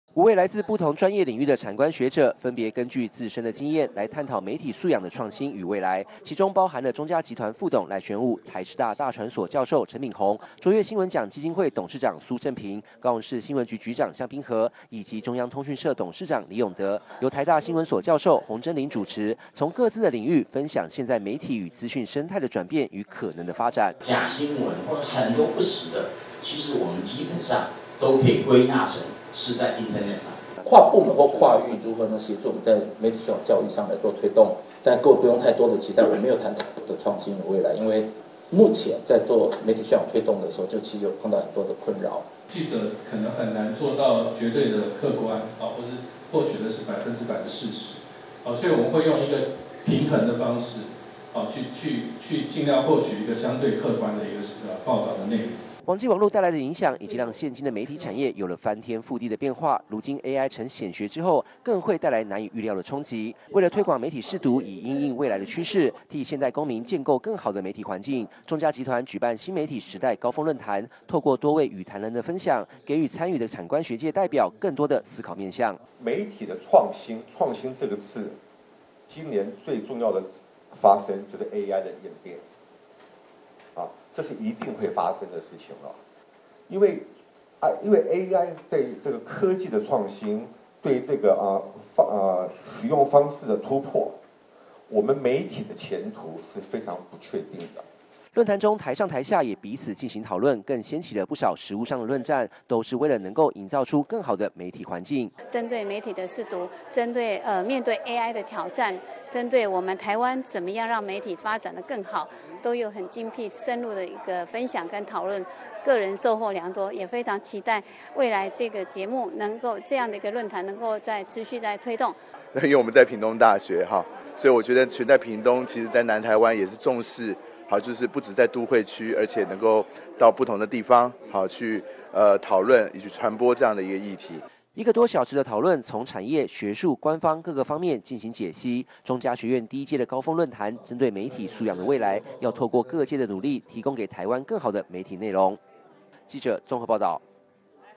媒體 論壇|中嘉媒體高峰論壇 尋求媒體素養創新
面對日新月異的傳播科技，傳播媒體該如何在AI新科技及媒體素養間取得平衡，中嘉集團「中嘉學院第一屆新媒體時代高峰論壇」於屏東舉行，率產業之先，聚焦人工智慧與媒體素養議題，現場邀請超過五十位產官學界領袖共同參與，共同探討當代數位公民的挑戰與責任。
來自全台多達50位，學有專精的產官學界代表齊聚一堂，參與中嘉集團舉辦的「中嘉學院第一屆新媒體時代高峰論壇」，針對媒體素養的創新與未來，特別是目前引發熱論的生成式AI興起，顛覆傳統媒體的發展，進行專業的對話，也是創產業之先，針對新媒體的未來進行探談。
在這個資訊容易產製的時代，生成式ai讓內容與影像的產製更加容易，不過也帶來許多未曾出現的倫理問題，中嘉集團一共要用兩天的時間，舉辦兩場口頭論文發表與一場論壇，要透過討論，深入探討每一項傳播科技發明帶來的挑戰與因應對策。
難得的是，不只是學界彼此交流，這次更透過業界的參與，還有官方的加入，三方交流彼此碰撞，期待能藉此激發出不同的火花。
面對日新月異的傳播科技，中嘉集團也面臨挑戰，同時為了善盡社會責任，更率先發起針對媒體演變的高峰論壇，幫忙找出當代數位公民的必備媒體識讀能力。
能夠有這樣的機會參與論壇討論，也讓不少到場教授都感到相當興奮，讓彼此都有學習討論的空間。
中嘉集團期盼以媒體的力量整合各方資源，拋磚引玉，將媒體素養的精神向下扎根，實踐媒體集團的社會責任，建構一個健全的媒體素養環境，為民主政治與公民素養立下根基。